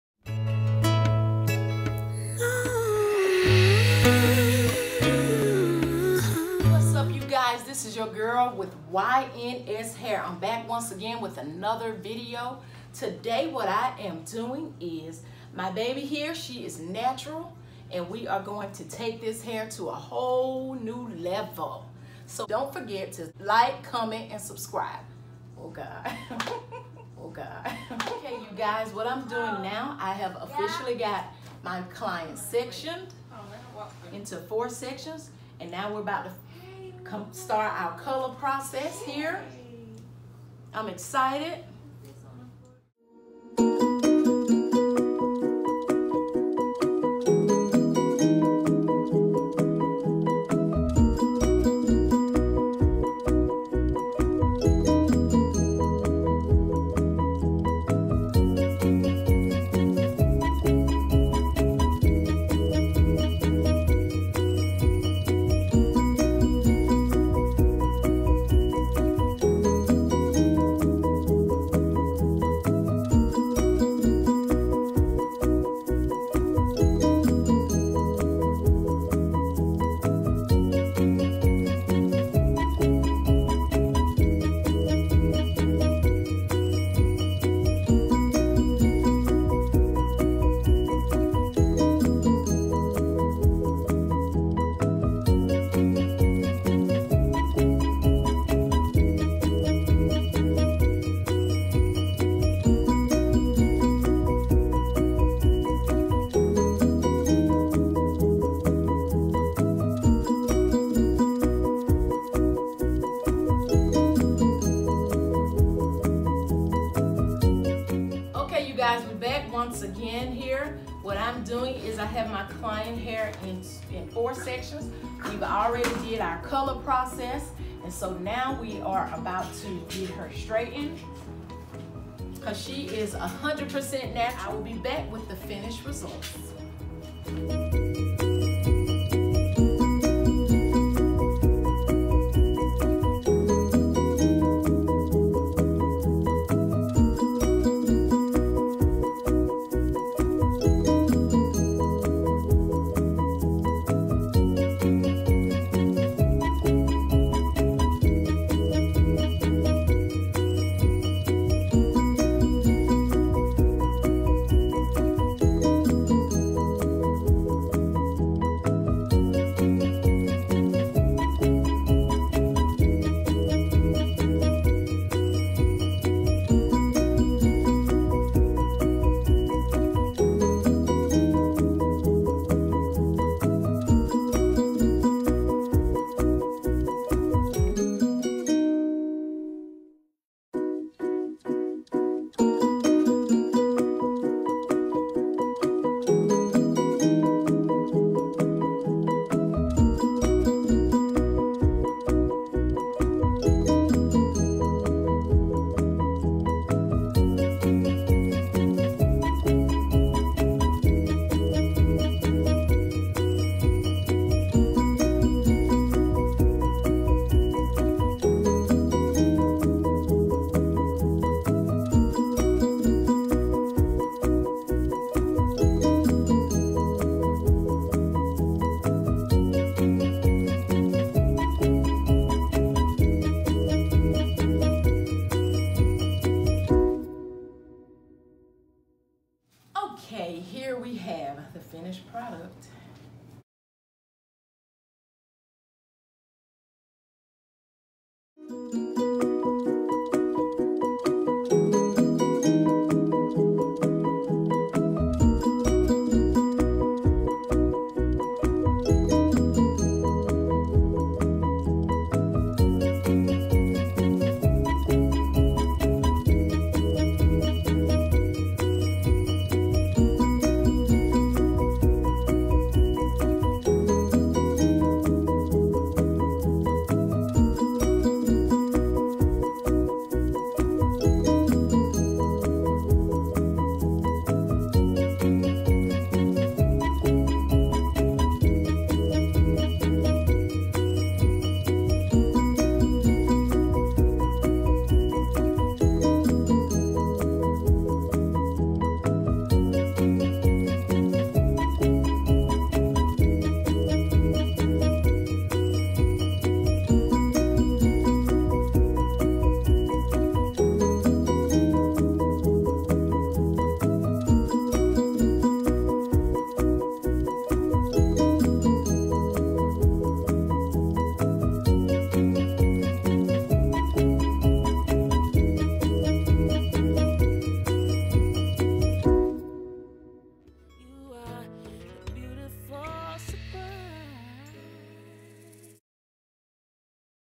HOW TO COLOR NATURAL HAIR (SAFELY)
Pre lighten Natural Hair 2 levels then apply toner of your choice.